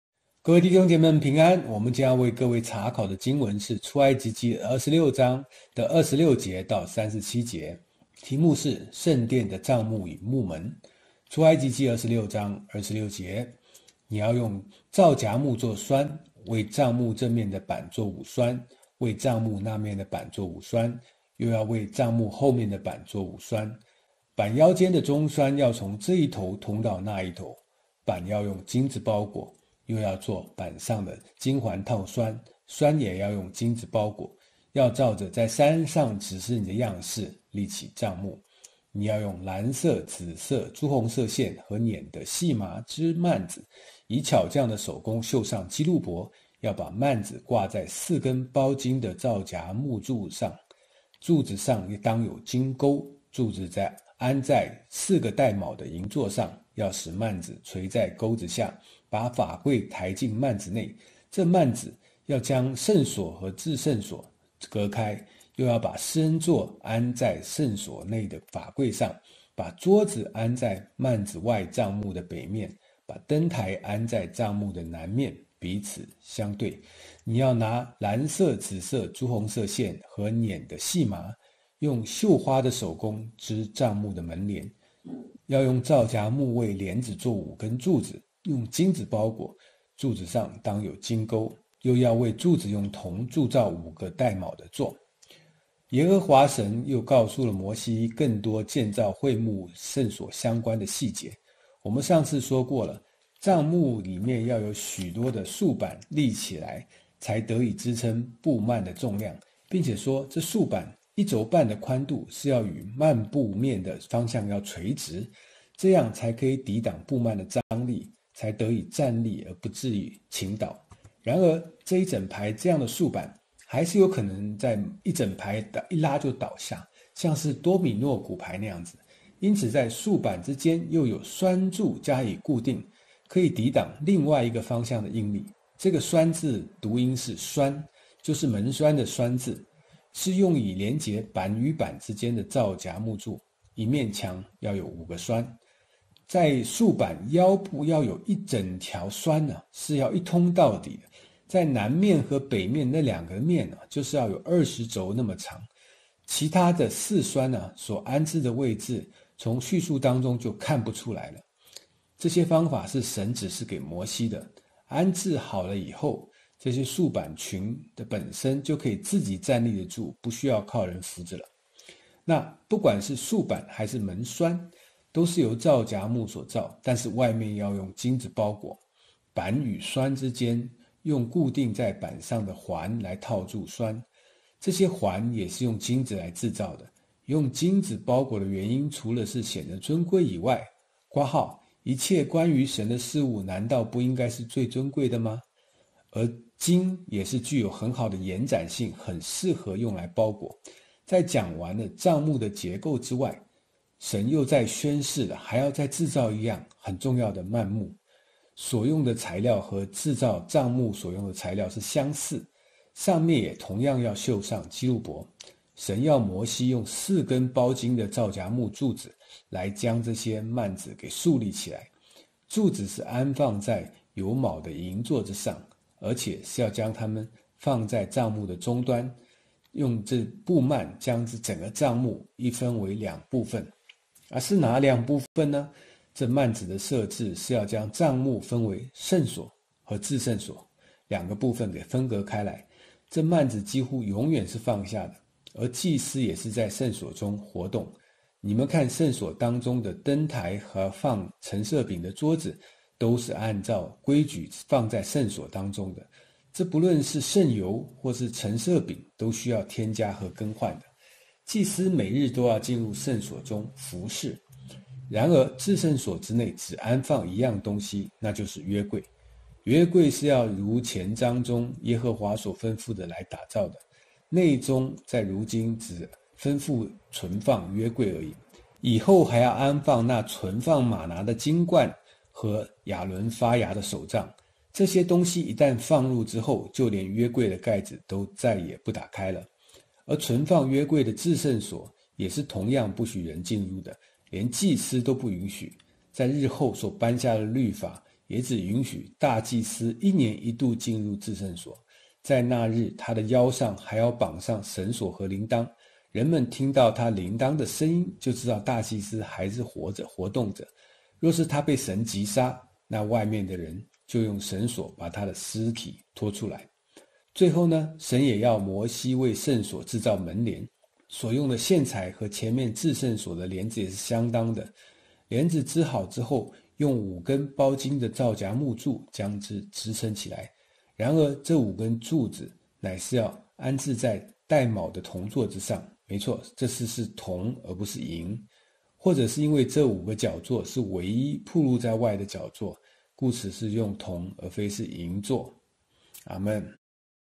聖殿的帳幕與幕門（出26:26）
出埃及記26章：26 你要用皂莢木做閂，為帳幕這面的板做五閂， 27 為帳幕那面的板做五閂，又為帳幕後面的板做五閂。 28 板腰間的中閂要從這一頭通到那一頭。 29 板要用金子包裹，又要做板上的金環套閂，閂也要用金子包裹。 30 要照著在山上指示你的樣式立起帳幕。

31 「你要用藍色、紫色、朱紅色線和撚的細麻織幔子，以巧匠的手工繡上基路伯。 32 要把幔子掛在四根包金的皂莢木柱子上，柱子上當有金鉤，柱子安在四個帶卯的銀座上。 33 要使幔子垂在鉤子下，把法櫃抬進幔子內，這幔子要將聖所和至聖所隔開。 34 又要把施恩座安在至聖所內的法櫃上， 35 把桌子安在幔子外帳幕的北面，把燈臺安在帳幕的南面，彼此相對。

36 「你要拿藍色、紫色、朱紅色線和撚的細麻，用繡花的手工織帳幕的門簾。 37 要用皂莢木為簾子做五根柱子，用金子包裹，柱子上當有金鉤，又要為柱子用銅鑄造五個帶卯的座。

耶和華神又告訴了摩西更多的建造會幕聖所的相關細節。我們上次說過了帳幕裡面要有許多的豎板立起來用以支撐布幔的重量，並且說了這豎板一肘半的寬度是要與幔布面的方向垂直，這樣才得以抵擋布幔的張力，才得以站立得住不至於傾倒。

然而一整排這樣的豎板，還是有可能整排一拉就倒下，像是多米諾骨牌那樣子。因此在豎板之間又有閂柱加以固定，可以抵擋另外一個方向的應力。這個閂字是讀「栓」，就是門閂的閂字，是用以連結板與板之間的皂莢木柱，一面牆要有五個閂。

在豎板腰部處要有一整條閂是要一通到底，在南面和北面的那兩個面就是要有20肘那麼長，其它的四閂所安置的位置從敘述當中就看不出來了，這些方法是神指示給摩西的，安置好了以後，這些豎板群本身就可以自己站立得住，不需要靠人扶著了。

不管是豎板還是閂柱，都是由皂莢木所造，但是外面要用金子包裹，板與閂之間是用固定在板上的環來套住閂，這些環也是用金子來製造。用金子包裹的原因除了是顯得尊貴之外（一切關於神的事物難道不應該是最尊貴的嗎？）而金也是具有很好的延展性，很適合用來包裹。

在講完了帳幕的結構之外，神又再宣示了要再製造另一樣重要的幔幕，所用的材料和制造帳幕所用的材料相似，上面也同樣的要繡上基路伯，神要摩西用四根包金的皂莢木柱子來將這些幔子給樹立起來，柱子是安放在有卯的銀座之上，而且是要將它們放在帳幕的中端，用這布幔將整個帳幕一分成為兩部分。

是哪兩部分呢？這幔子的設置是要將帳幕分為聖所和至聖所兩個部分給分隔開。這幔子幾乎永遠是放下的，而祭司也是在聖所中活動，你們看聖所當中的燈台和放陳設餅的桌子、都是按照規矩放在聖所當中，這不論是聖油 和陳設餅都需要添加和更換的。祭司每日都要進入聖所中服事。

然而至聖所之內只安放一樣東西，那就是約櫃；約櫃就是要如前章中耶和華所吩咐的來打造的，內中在如今只吩咐要存放法板，以後還要安放那存放嗎哪的金罐和亞倫發芽的手杖。這些東西一但放入之後就連約櫃的蓋子都再也不打開了。

而存放約櫃的至聖所也同樣是不許人進入的，連祭司都不允許；在日後所頒下的律法也只允許大祭司一年一度的進入至聖所，在那日他的腰上還要綁上繩索和鈴鐺，人們聽到他鈴鐺的聲音就知道大祭司還活著，若是他被神擊殺，那外面的人就用根繩子把他的屍體拖出來。

最後呢、神也要摩西為聖所製造門簾，所用的線材和前面的至聖所的簾子也是相當，簾子織好之後，用五根包金的皂莢木柱將之支撐起來，然而這五根柱子乃是要安置在帶卯的銅座之上。沒錯，這次是銅而不是銀，或者是因為這五個腳座是唯一暴露在外的腳座，故此用銅而非銀座。